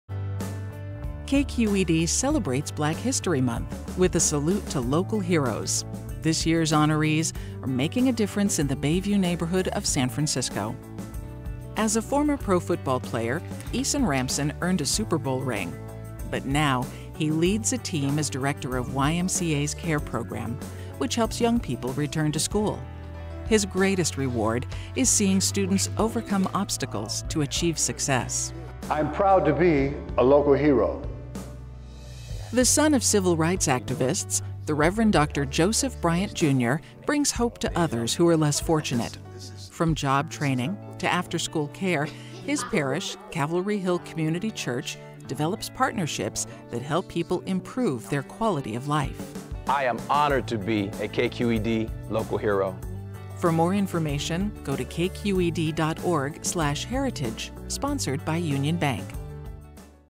KQED 2015 Black History Month Heroes Eason Ramson and Rev. Dr. Joseph Bryant, Jr
KQED honors Black History Month Local Heroes Eason Ramson, Director of CARE Program, YMCA SF Bayview Hunters Point; and
Rev. Dr. Joseph Bryant, Jr, Senior Pastor, Calvary Hill Community Church.